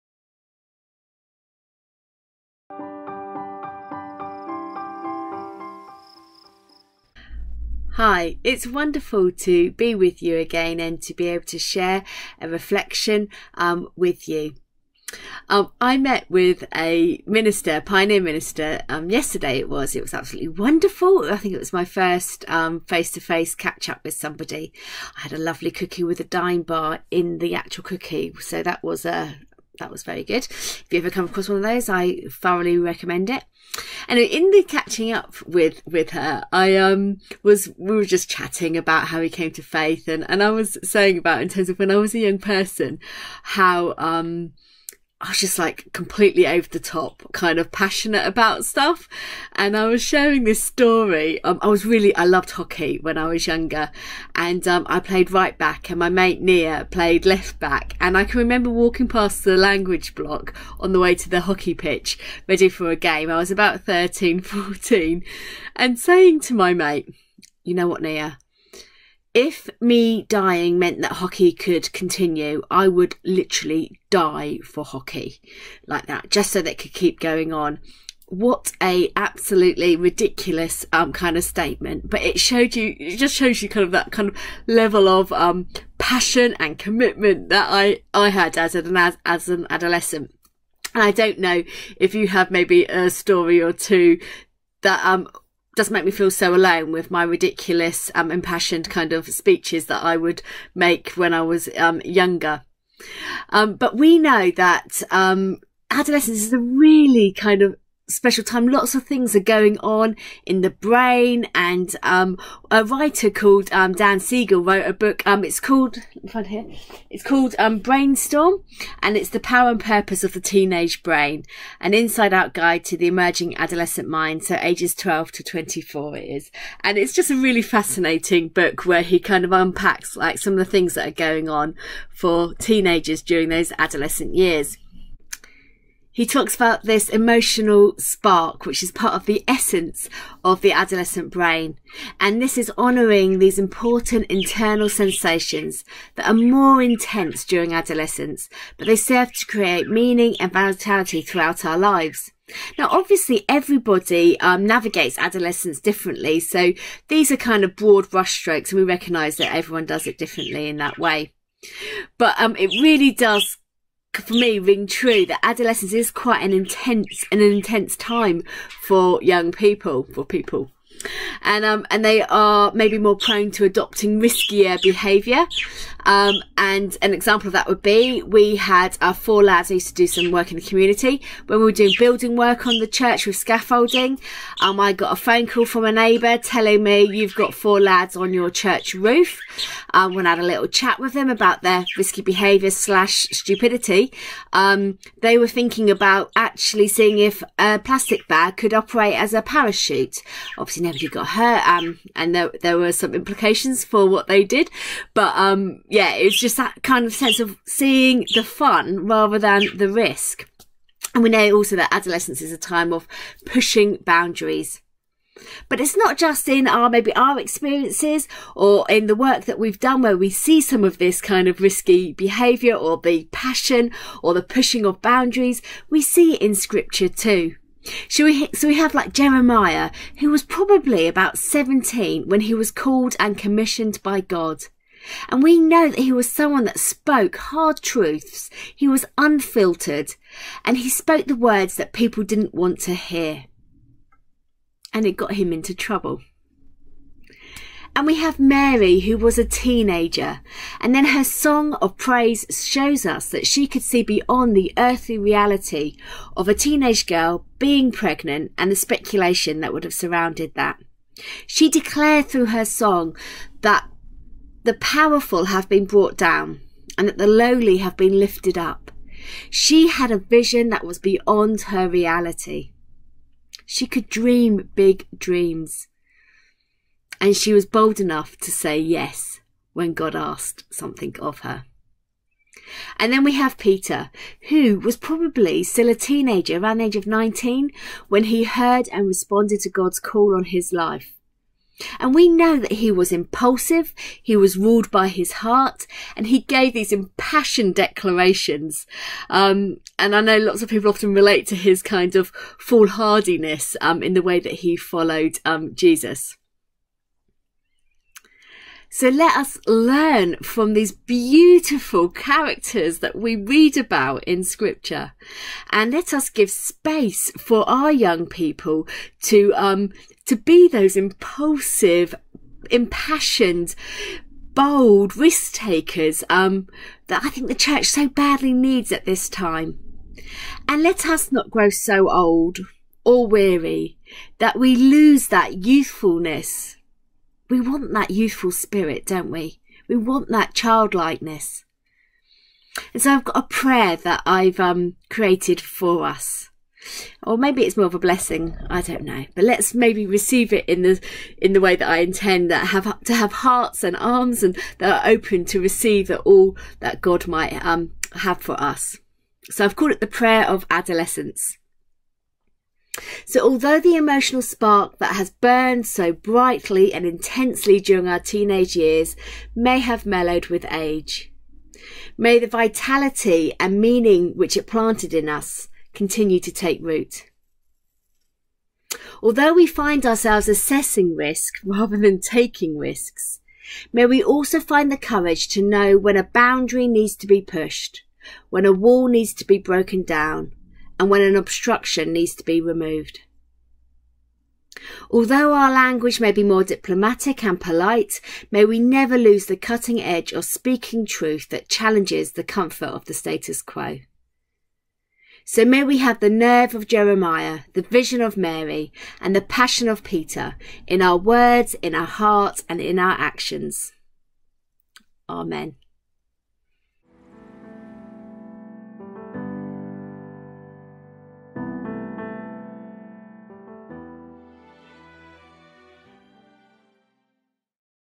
The Call of Adolescence
There is something special and precious about the risk taking of adolescence that we can easily lose later in life. Clare Hooper, SCBA Regional Minister for Children, Youth and Families reflects on the value of youthfulness, how God can use impulsive outspoken risk takers and how we might rediscover our own youthfulness .